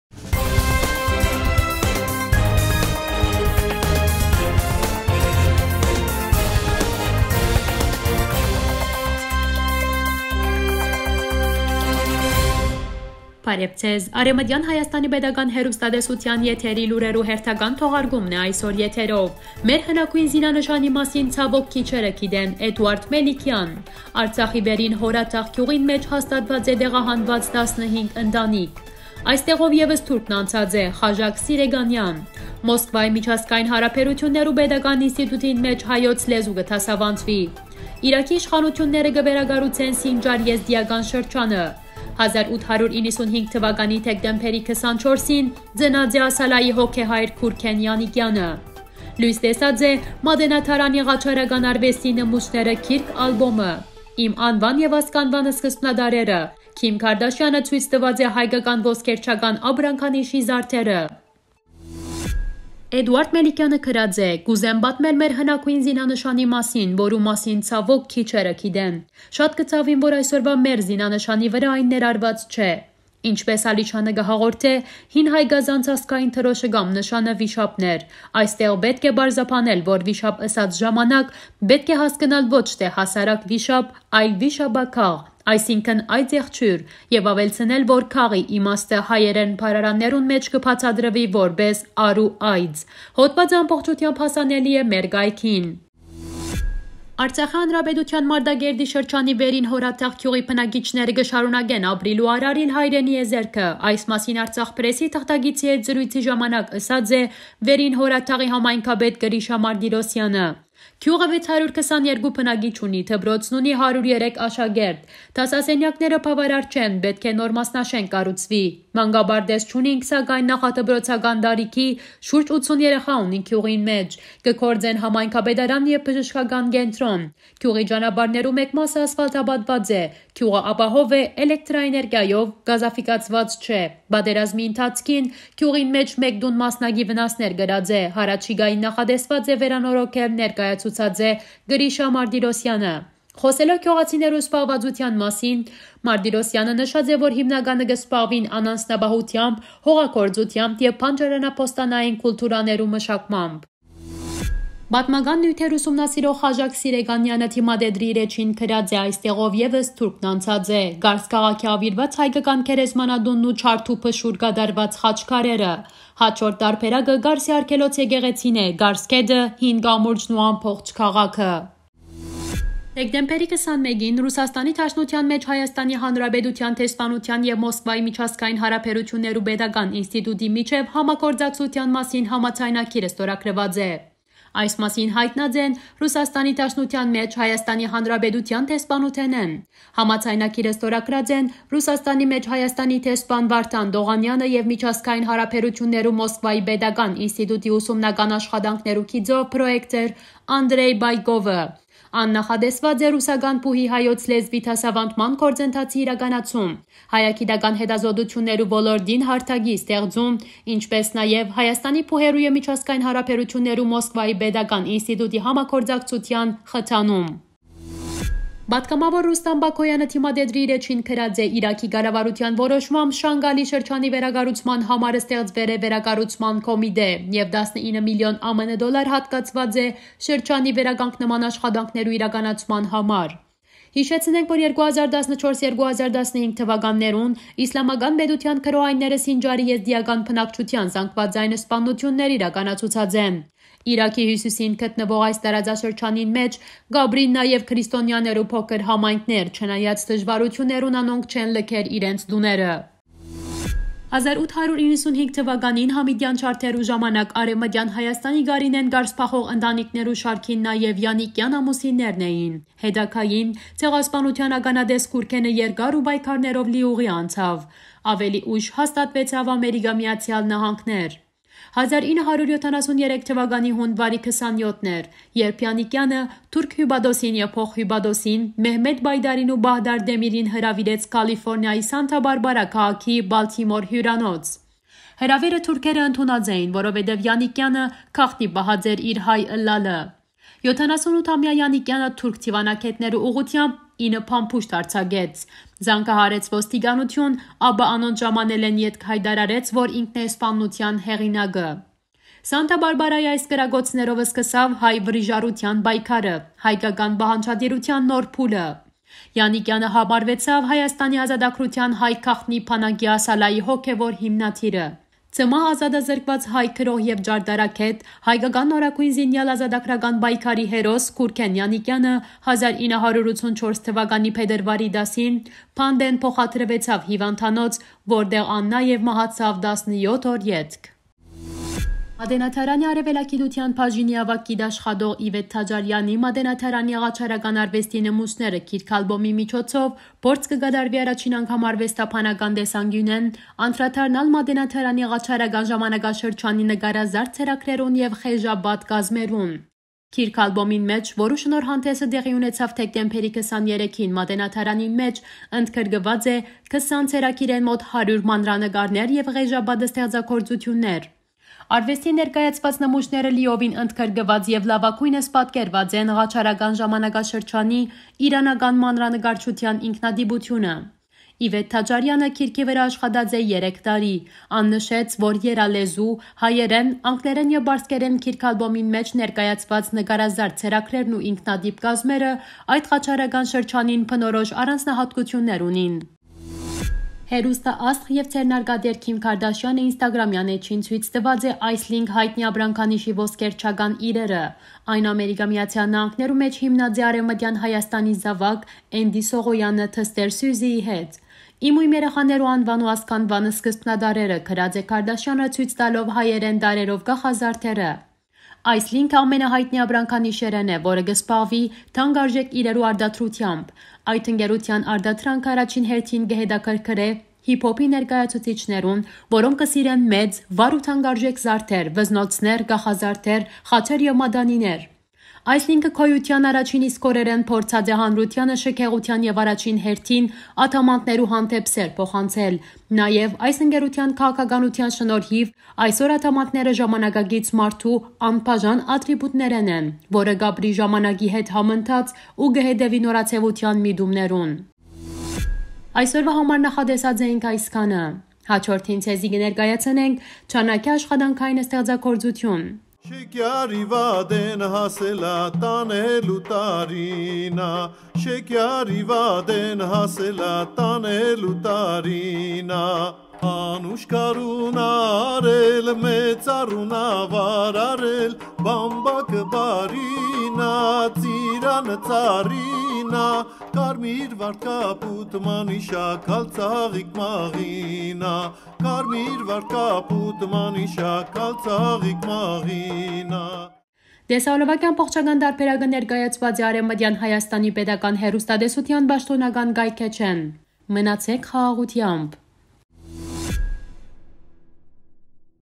1895 թ.Դեկտեմբերի 24-ին ծնած է ԱՍԱԼԱ-ի հոգեհայր Գուրգեն Եանիկեանը
1.«Մեր հնագոյն զինանշանի մասին, ցաւօք քիչերը գիտեն». Էտուարտ Մելիքեան
2.Արցախի Վերին Հոռաթաղ գիւղին մէջ  հաստատուած է տեղահանուած 15 ընտանիք. Համայնքապետ
3.«Այստեղով եւս թուրքն անցած է». Խաժակ Սիրեկանեան
4.Մոսկուայի միջազգային յարաբերութիւններու պետական ինստիտուտին մէջ հայոց լեզու կը դասավանդուի
5.Իրաքի իշխանութիւնները կը վերակառուցեն Սինջար եզտիական շրջանը
6.1895 թ.Դեկտեմբերի 24-ին ծնած է ԱՍԱԼԱ-ի հոգեհայր Գուրգեն Եանիկեանը
7.Լոյս տեսած է «Մատենադարանի ղաջարական արուեստի նմուշները» գիրք-ալպոմը
8.«Իմ անուան եւ ազգանուան սկզբնատառերը». Քիմ Քարտաշեանը ցոյց տուած է հայկական ոսկերչական ապրանքանիշի զարդերը